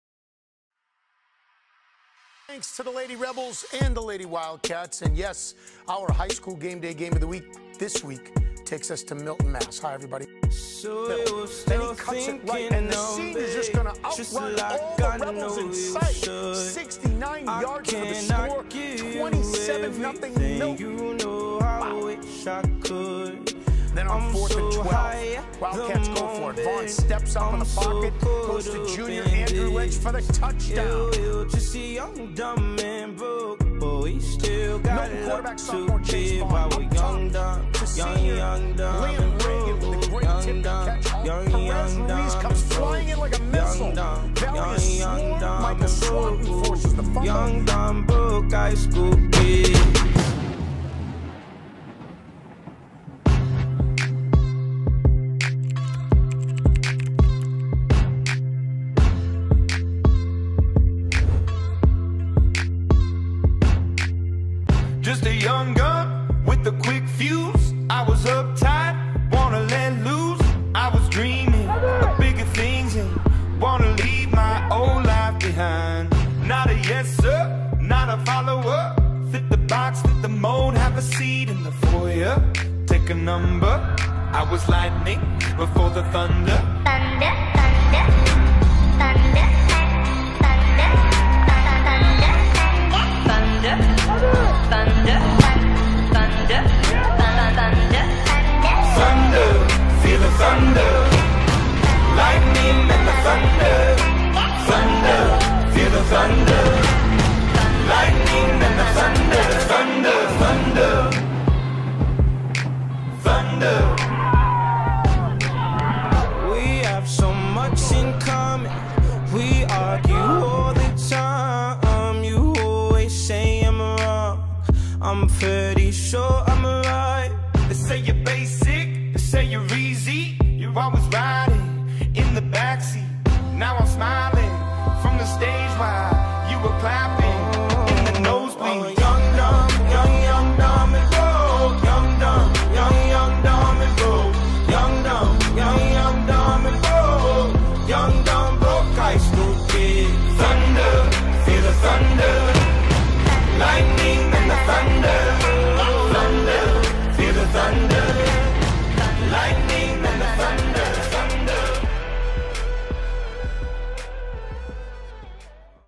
MHS WILDCATS FOOTBALL 2019
A montage style video featuring clips from the Milton Vs Weymouth football game.

Shot on Sony A7RIV w/ Movi Pro
Drone Used DJI Inspire 2 X7